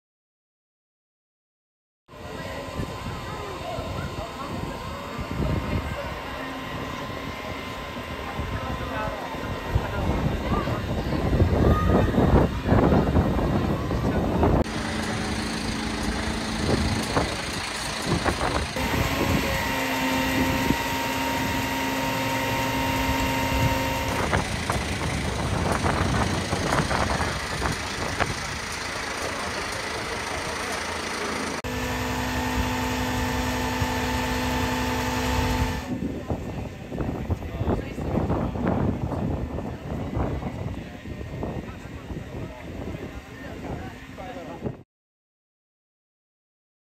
Firefighters at York Minster
They were testing out this amazing new aerial platform.